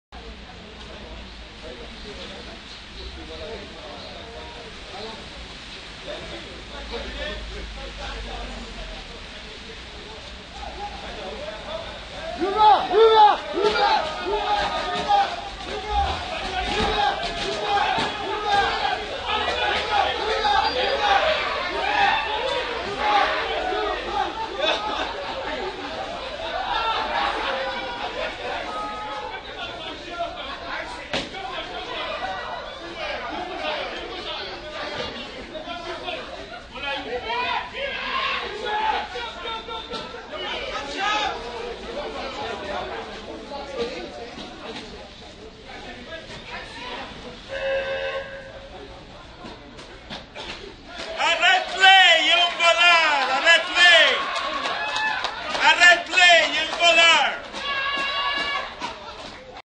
Des chauffeurs de taxi énervé contre un Uber
La scène se  passe à Montréal
Vidéo original: